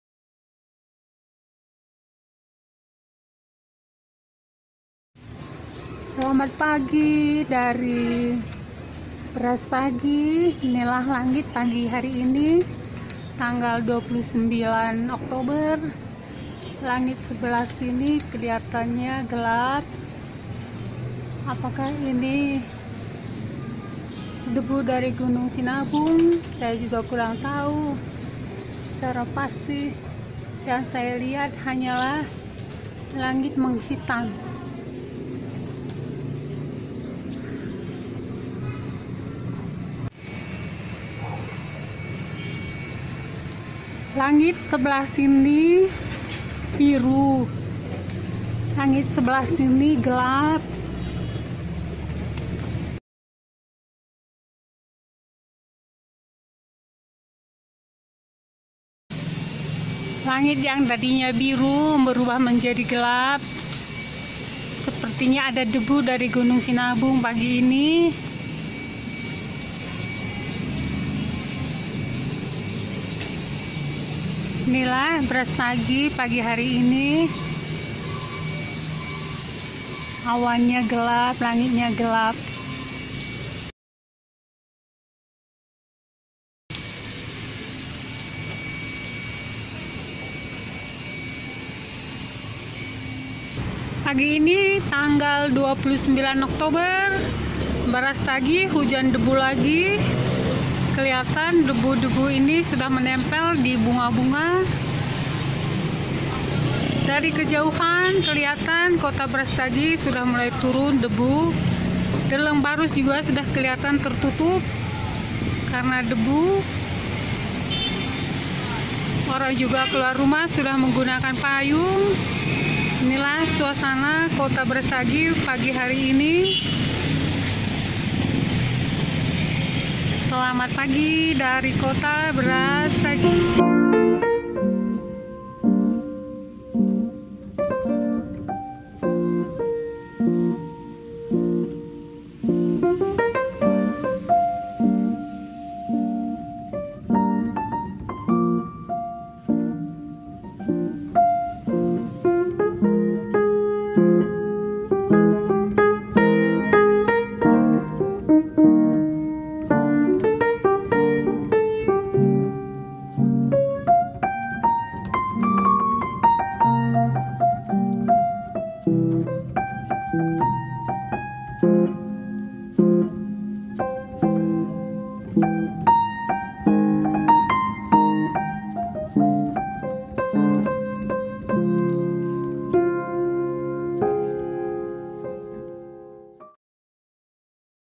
Sinabung Erupsi (Lagi) Pagi Ini - Kota Berastagi Hujan Abu Vulkanik
Sinabung Erupsi Langit Berastagi Menghitam.
Reported by Susan Ginmun
Video Voice by Susan Ginmun
YouTube Studio Music
Live Delay from Berastagi,Oct 29,2020.
Kaset Mekaro Arsisu Video Official

Thanks 4 watching, Mejuah-juah...
YouTube Kaset Mekaro Arsisu